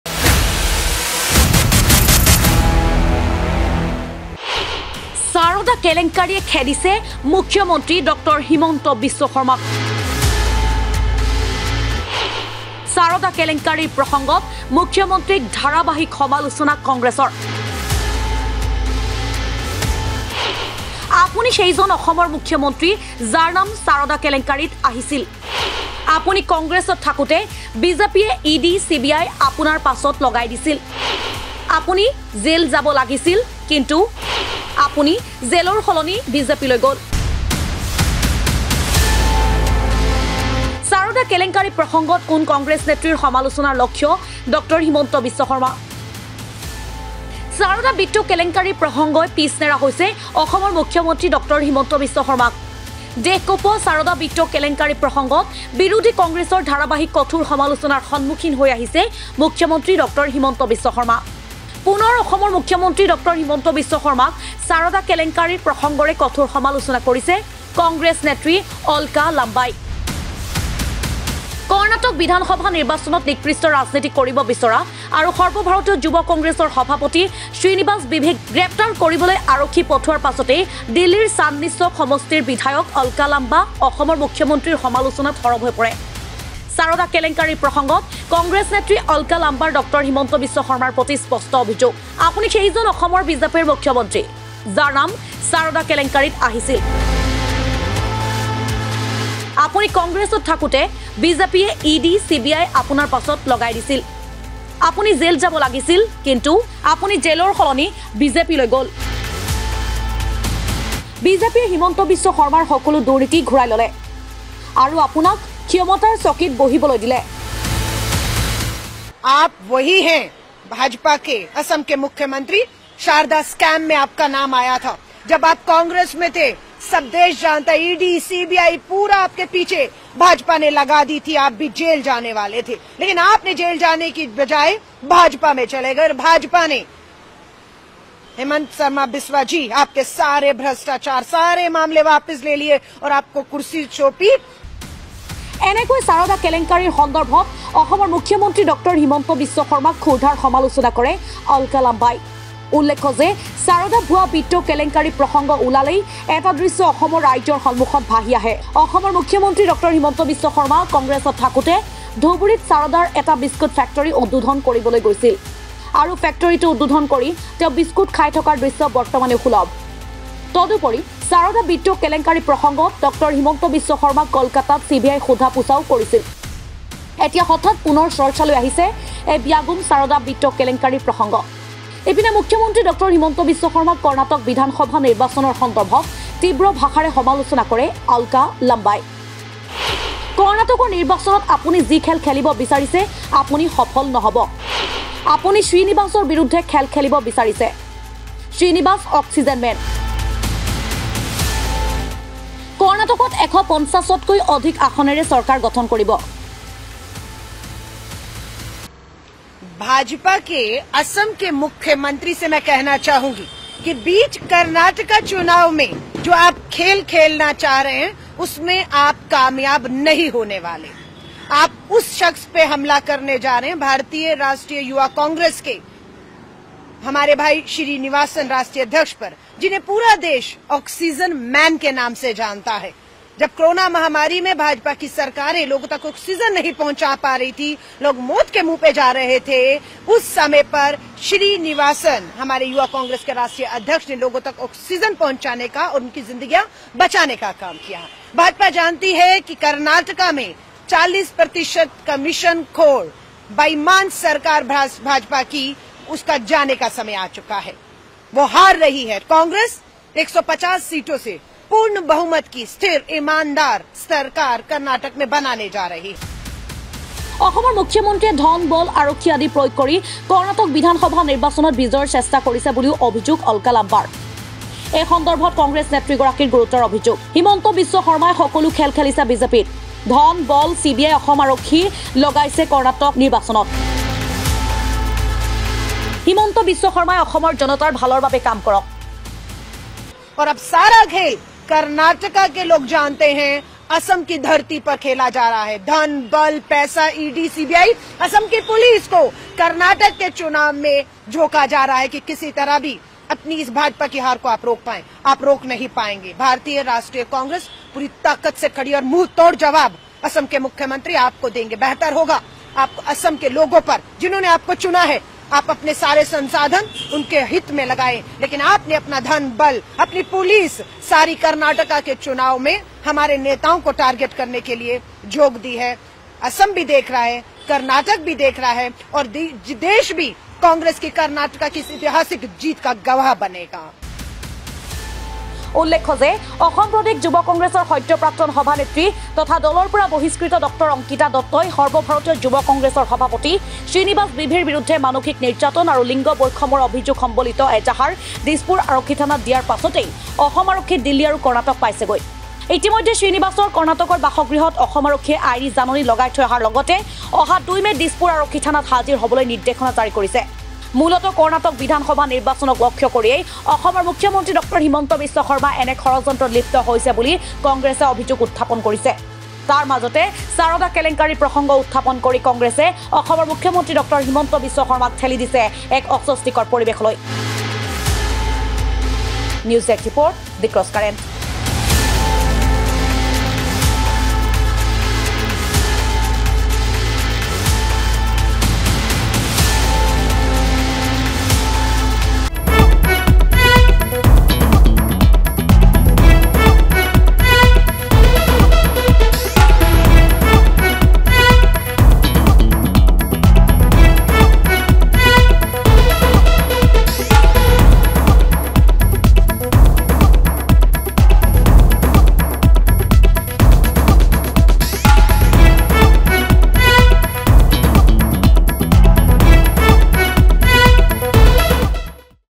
সাৰদা কেলেংকাৰীয়ে খেদিছে মুখ্যমন্ত্ৰী হিমন্তবিশ্ব শৰ্মাক
সাৰদা কেলেংকাৰীয়ে খেদিছে মুখ্যমন্ত্ৰী হিমন্তবিশ্ব শৰ্মাক। সাৰদা কেলেংকাৰীৰ প্ৰসংগত মুখ্যমন্ত্ৰী হিমন্ত বিশ্ব শৰ্মাক ধাৰাবাহিক সমালোচনা কংগ্ৰেছৰ। সাৰদা কেলেংকাৰী প্ৰসংগত কোন কংগ্ৰেছ নেত্ৰীৰ  সমালোচনাৰ লক্ষ্য ড° হিমন্তবিশ্ব শৰ্মা?